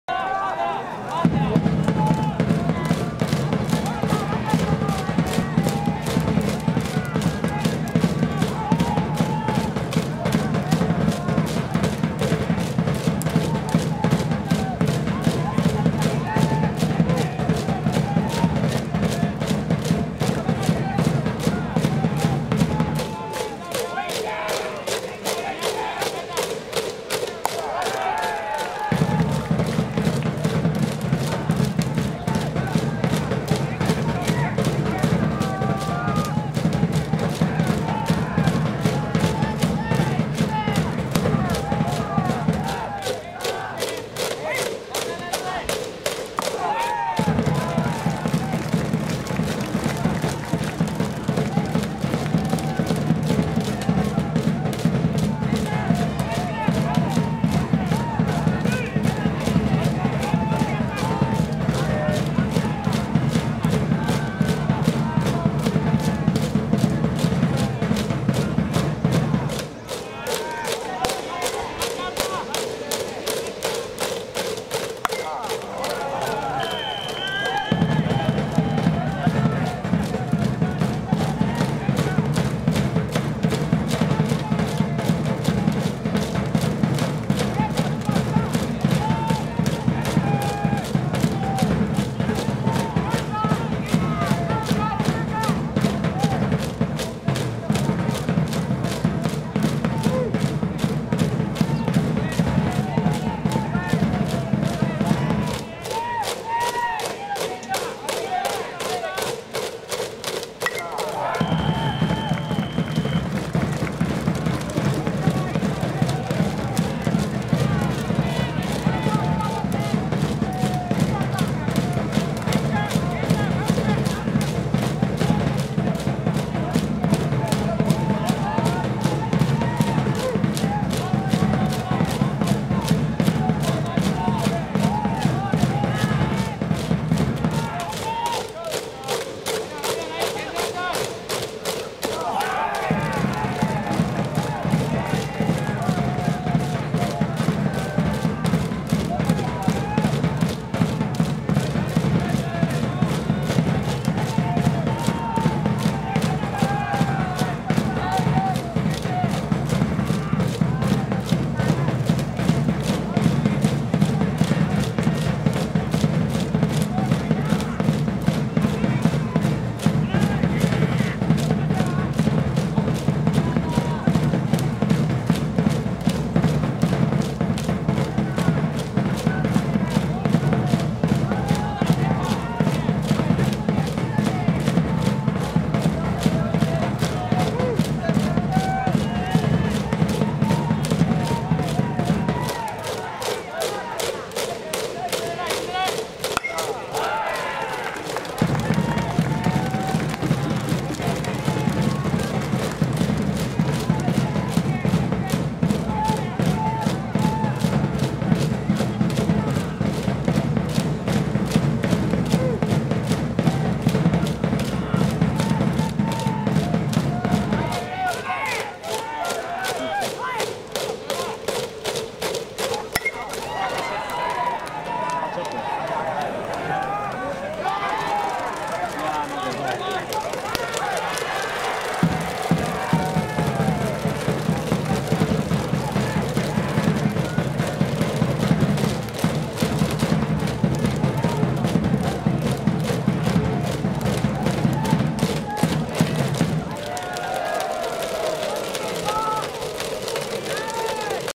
鳴門高校 土肥憲将 犠飛で追加点 ＠むつみスタジアム 20220723
2022年7月23日（土）　第104回全国高校野球選手権徳島大会　準々決勝
鳴門７-３富岡西　むつみスタジアム（蔵本）